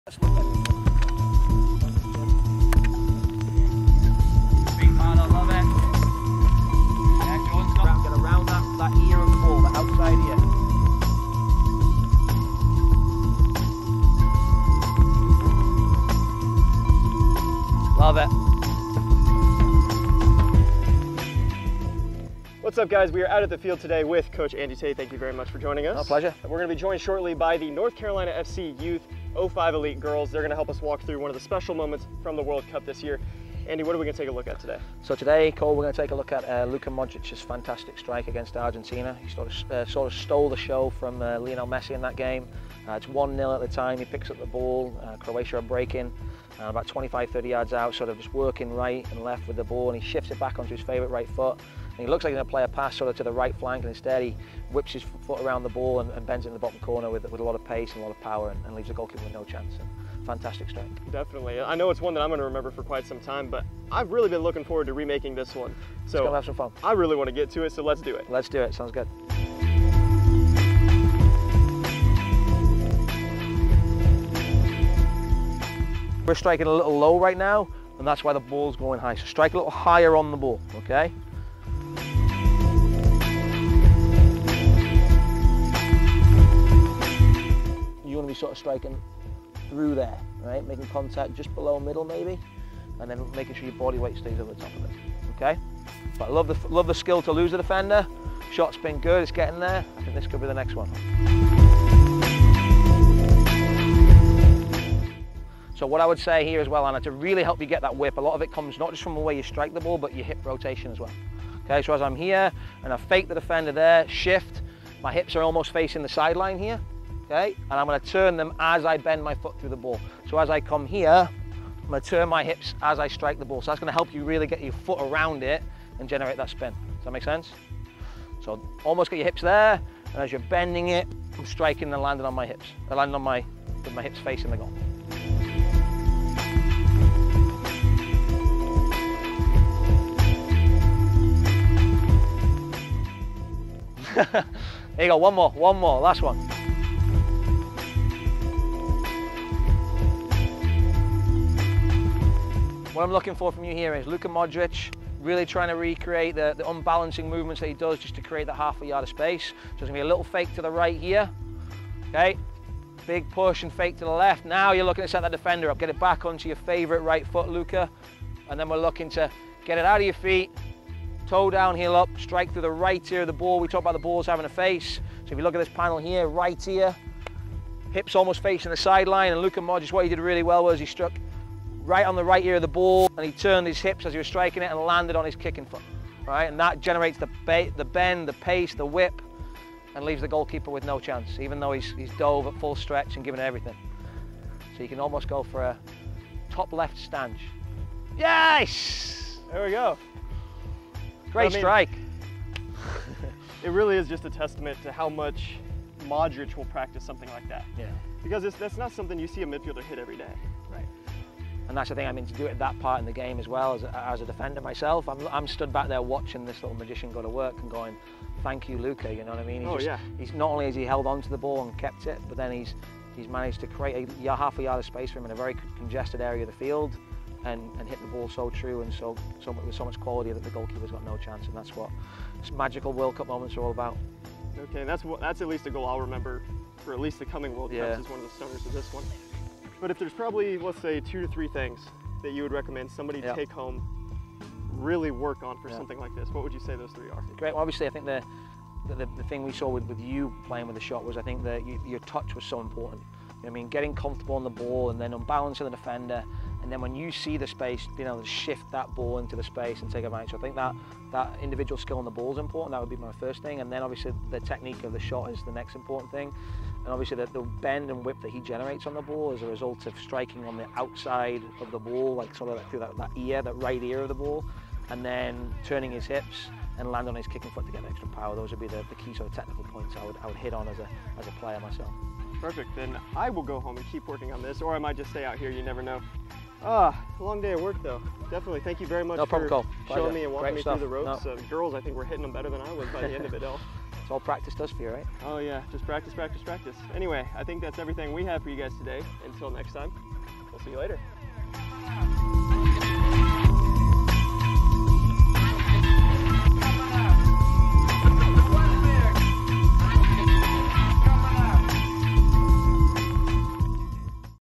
FIFA World Cup™ Skills with NCFC Youth: Create space and score like Luka Modric's
To help those aiming to CelebrateNext and become a future FIFA World Cup™ star, we teamed with the first-class coaches and players at North Carolina FC Youth.

Each week we’ll take a goal, move, technique or tactic from a match that caused mouths to drop and use it to help skills to sharpen.

When Luka Modrić lined up a stunning strike, scoring the second goal against Argentina, we just knew that was a goal we needed to master. So, to accomplish this, we met up with coach Andy Tate and the ’05 Elite Girls NCFC Youth team to work on our skills.